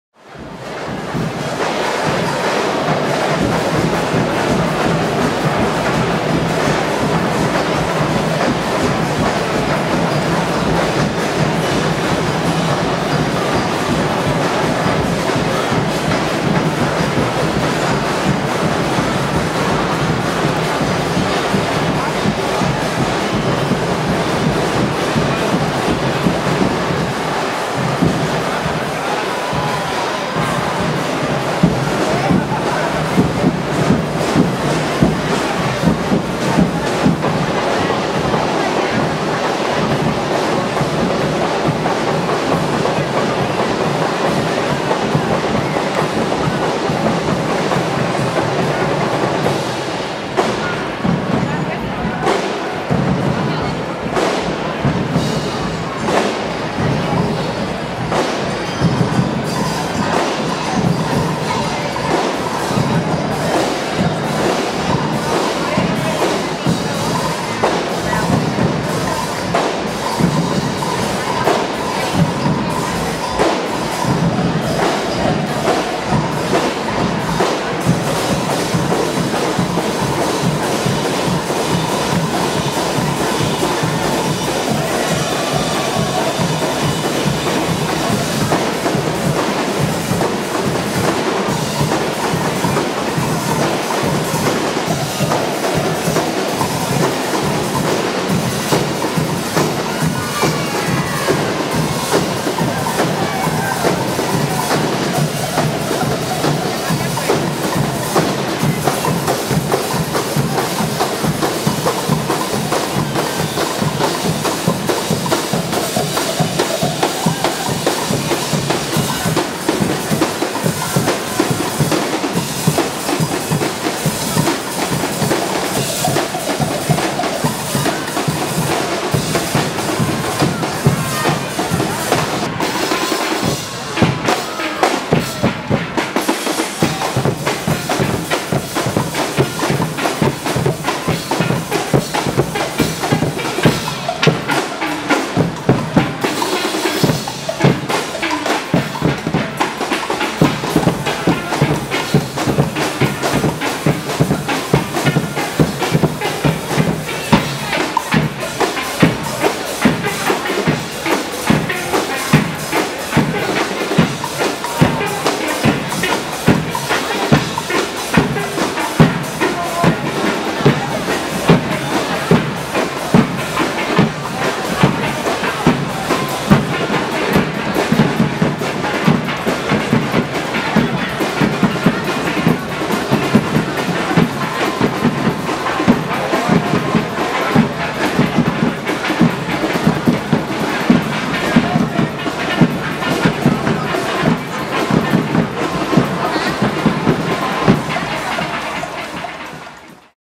Los Tukanes (24) - Desfile de comparsas 2014 Carnaval de Badajoz
Comparsa Los Tukanes en el desfile de comparsas del Carnaval de Badajoz 2014 el domingo 2 de marzo de 2014.

Premios:
- 12º accésit desfile de comparsas